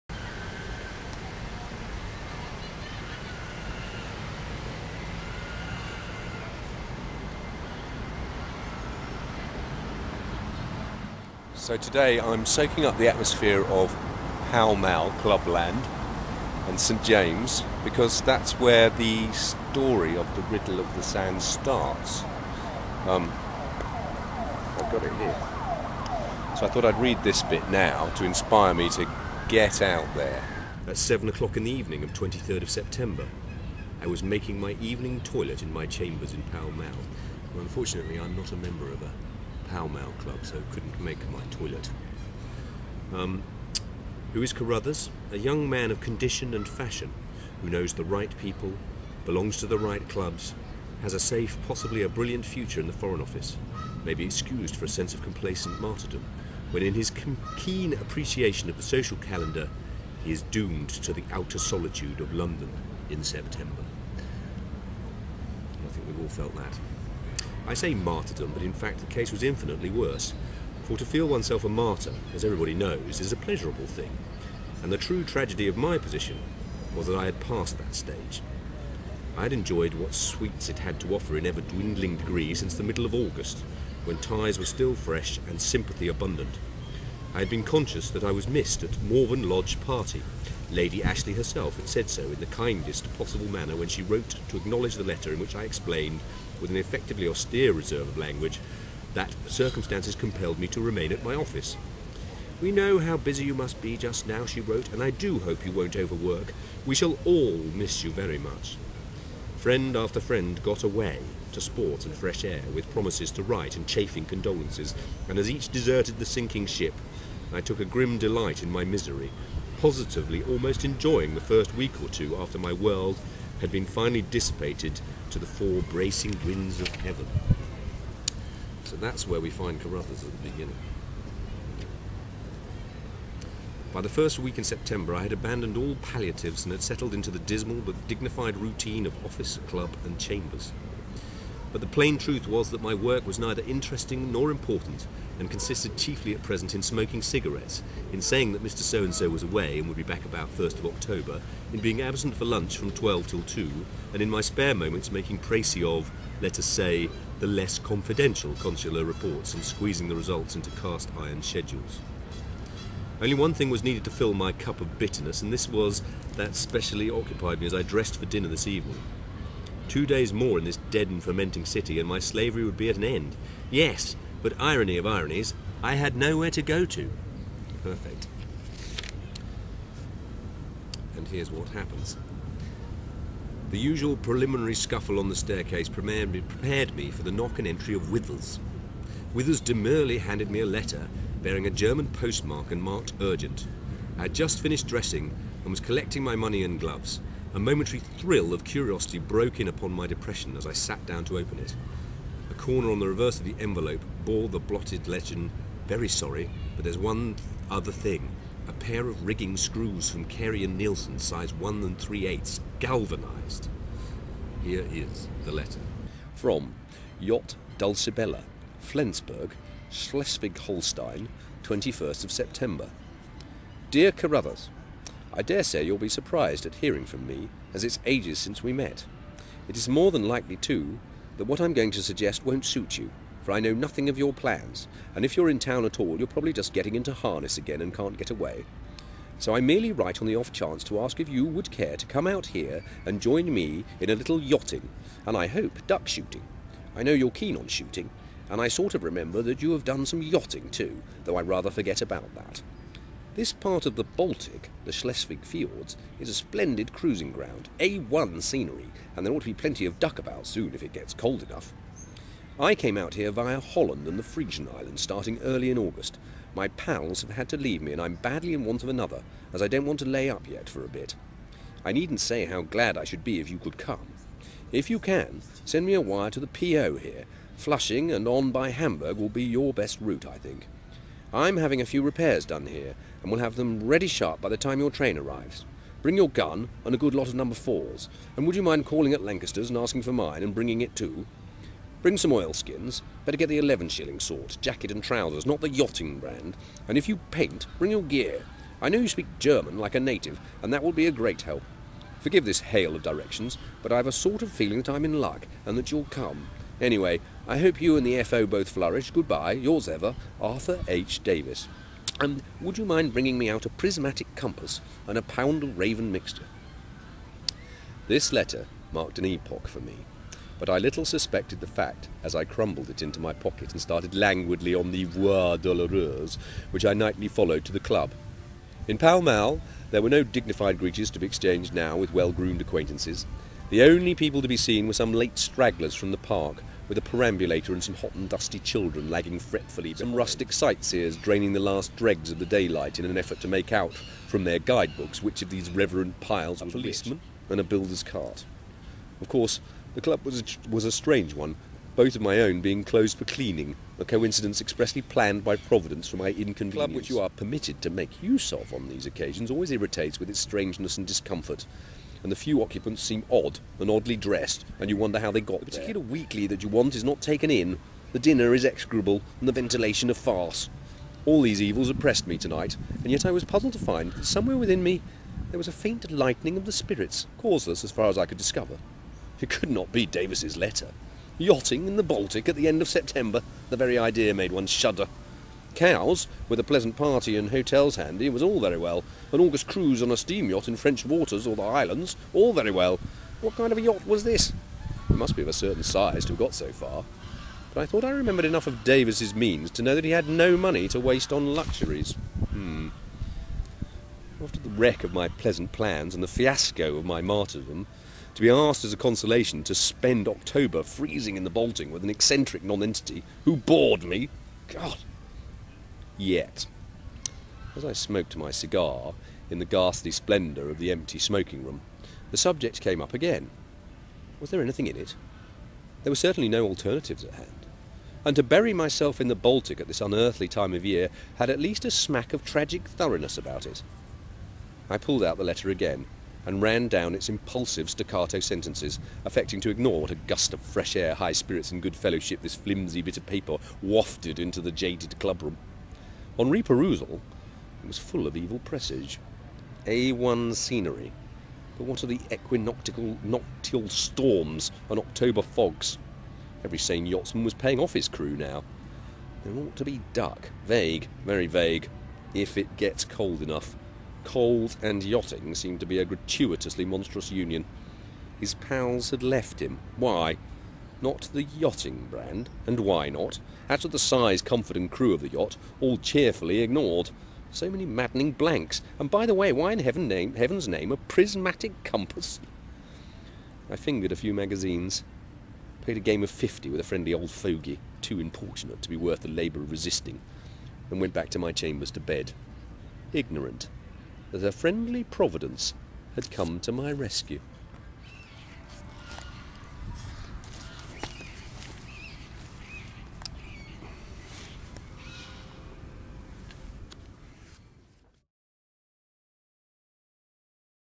the Kidmapper returns: bookmapping Riddle of the Sands
Having proved the point with Kidmapped! that some novels really can be played out in real time & 'geolocated', I've been on the lookout for other titles that can be 'bookmapped'.

Next week (19th -26th Oct) I'm off to Holland & Northern Germany to scope out the landscape described in Erskine Childers' classic 1903 spy thriller 'The Riddle of the Sands'.

or on Twitter (I am @moongolfer) - or you could come and join me! Full itinerary is on the blog.